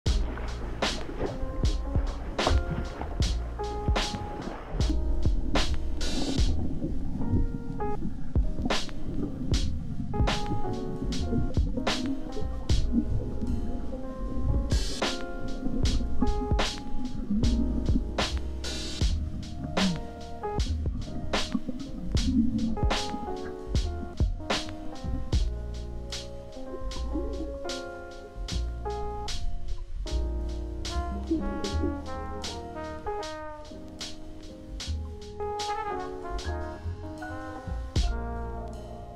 Underwater Walleye! Releasing Tournament Fish
Releasing tournament walleye back to the lake!